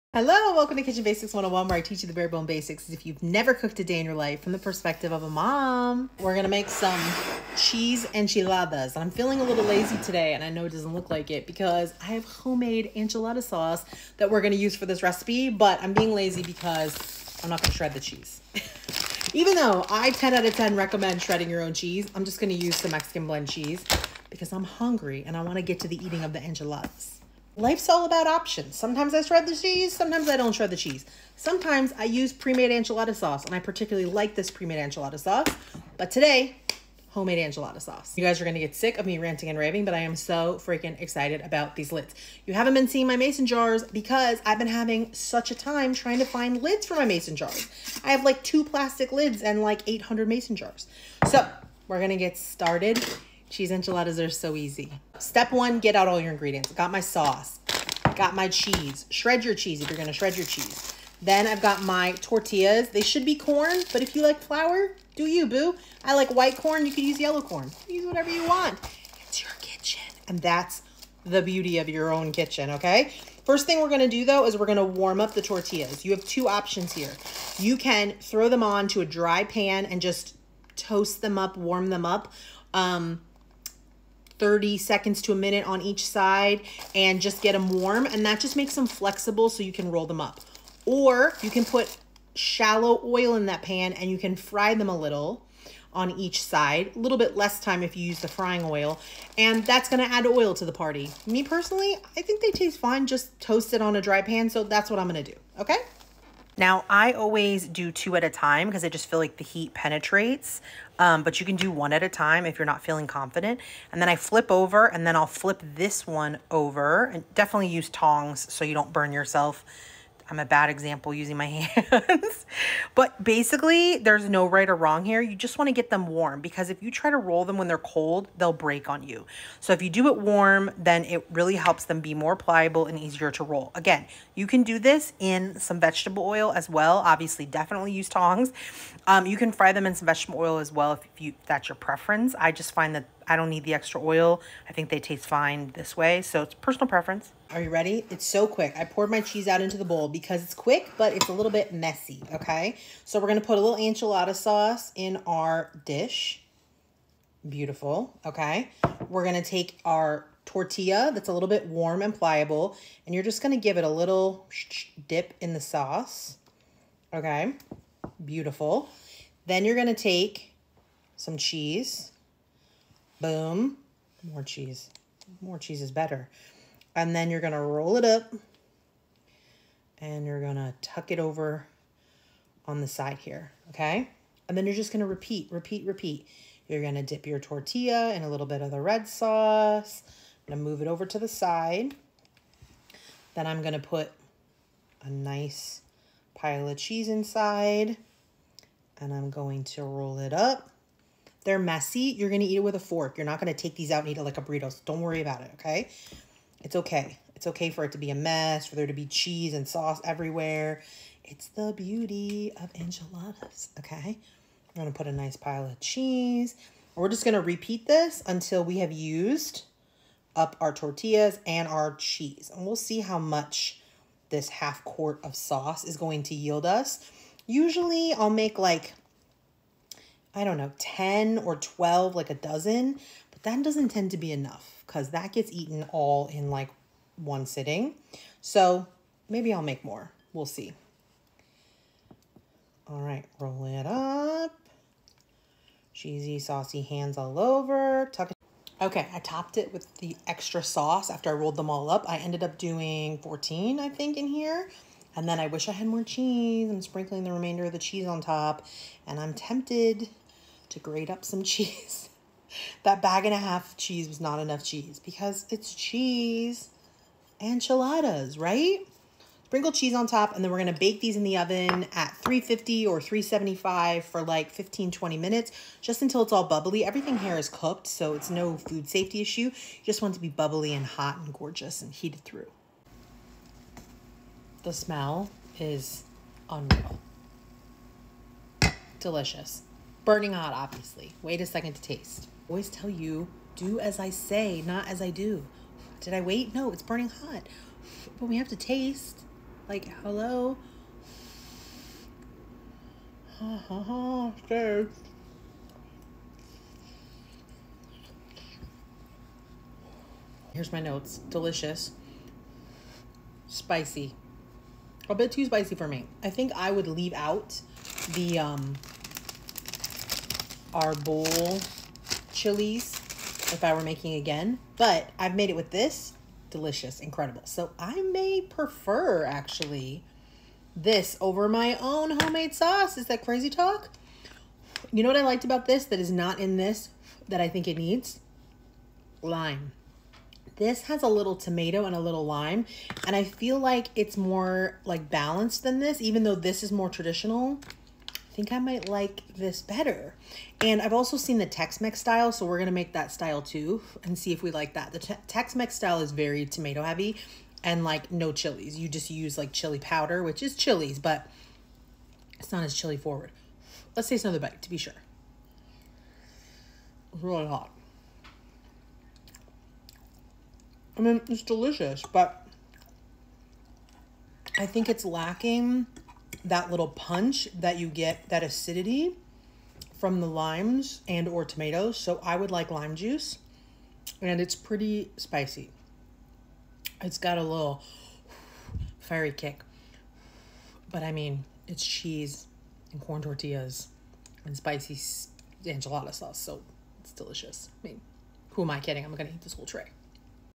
Deliciously Easy Homemade Cheese Enchiladas Recipe | Homemade Sauce and Mason Jar Lid Hack
Indulge in the ultimate comfort food with this deliciously easy homemade cheese enchiladas recipe! In this Kitchen Basics 101 tutorial, we'll guide you step by step through crafting these mouthwatering enchiladas from scratch, complete with a homemade sauce that's sure to elevate your dish to the next level. Plus, discover a clever mason jar lid hack to streamline your cooking process.

Join us in the kitchen as we teach you the bare bone basics of cooking, perfect for beginners and seasoned home chefs alike. Whether you're craving a satisfying weeknight dinner or planning a fiesta with friends, these cheese enchiladas are sure to be a hit.

So grab your ingredients and get ready to impress your taste buds. With our easy-to-follow instructions and helpful tips, you'll be whipping up restaurant-quality enchiladas in no time. Don't settle for store-bought when you can create homemade goodness right in your own kitchen!